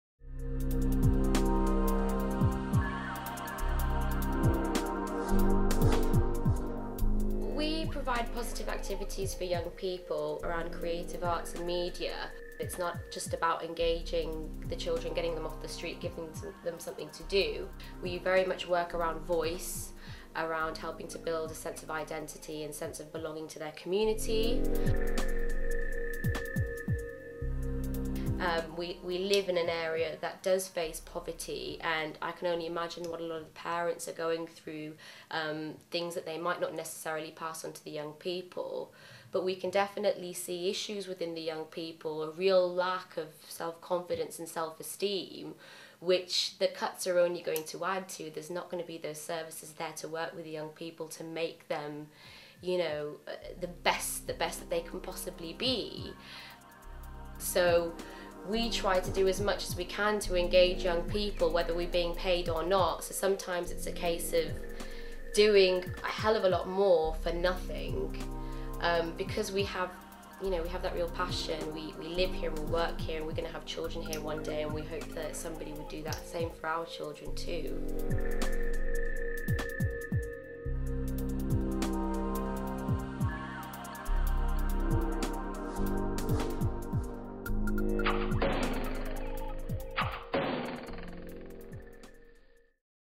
Robin On The Road Meets Union Street Media Arts
Union Street is a unique organisation combining a commercial media and creative arm - film, photography, graphic design and other forms of creative arts - with a community development project in an innovative way. The two sides to the business are separate but overlap frequently.

Being an enterprise and community resource, we welcome the opportunity to help people take on exciting challenges and new ideas.